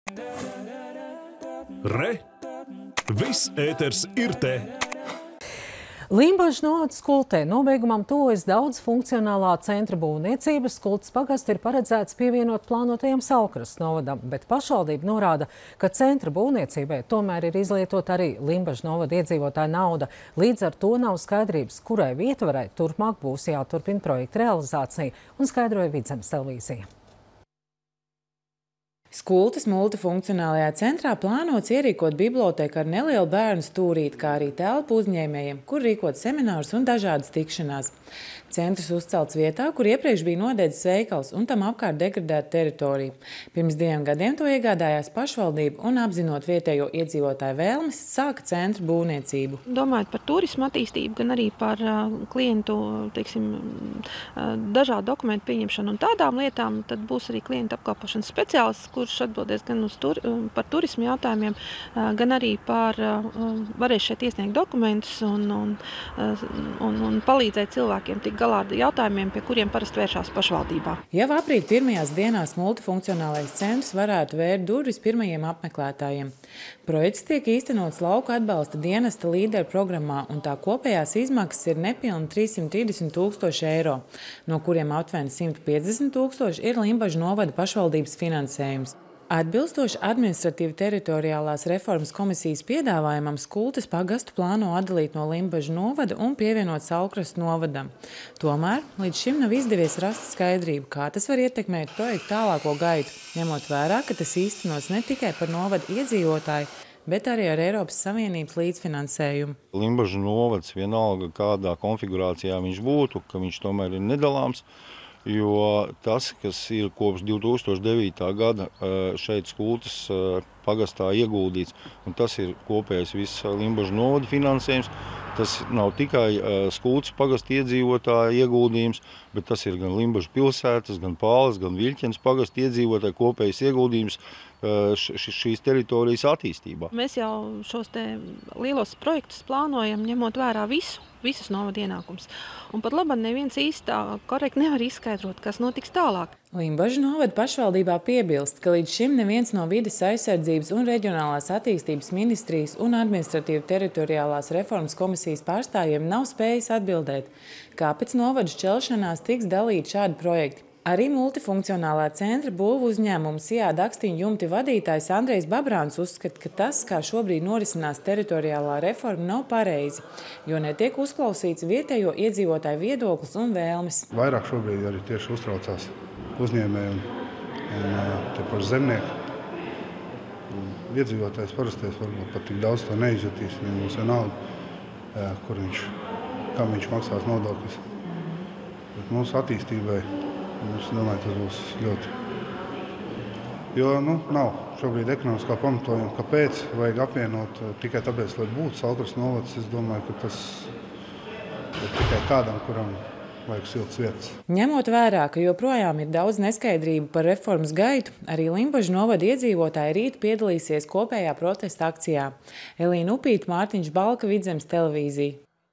Neskaidrības par projekta finansēšanu pēc reformas
Limbažu novada Skultē nobeigumam tuvojas daudzfunkcionālā centra būvniecība. Skultes pagastu ir paredzēts pievienot plānotajam Saulkrastu novadam. Pašvaldība norāda, ka centra būvniecībai tomēr ir izlietota arī Limbažu novada iedzīvotāju nauda. Līdz ar to nav skaidrības, kurai vietvarai turpmāk būs jāturpina projekta realizācija. Skaidroja Vidzemes TV.